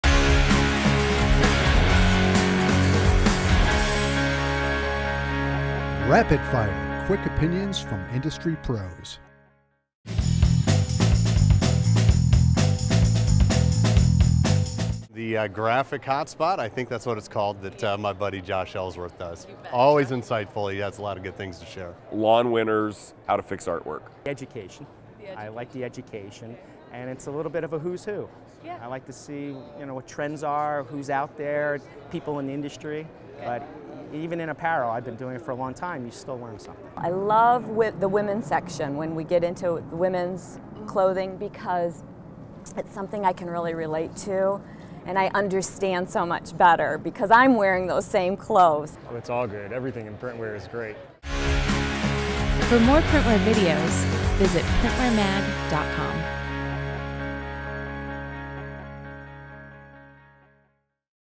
Rapid Fire: Favorite PW Feature
Industry experts tell us what their favorite feature is in each monthly issue of Printwear Magazine.